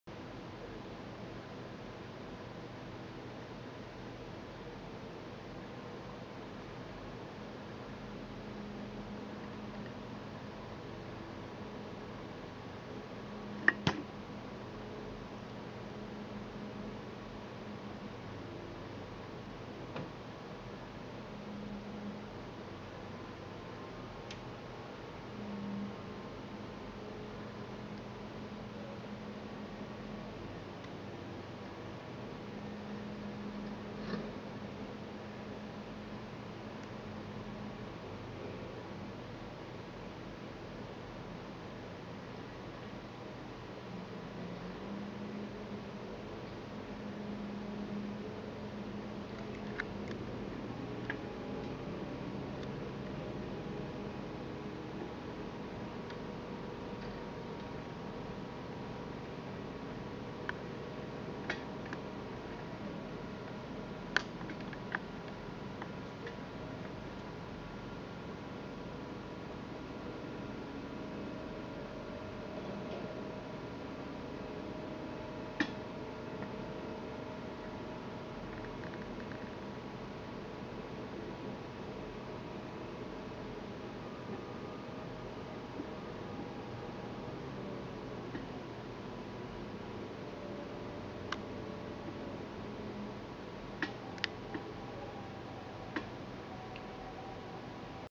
Dismantling Of The Dry Cleaning Factory - 11-06-2013 - 1
Ourania - Dismantling Of The Dry Cleaning Factory, At Last! - 10.25
And Again, They Did Not Told The Residents About It & About The Annoyance!
What Will Happen Next After This Noise & Obstruction?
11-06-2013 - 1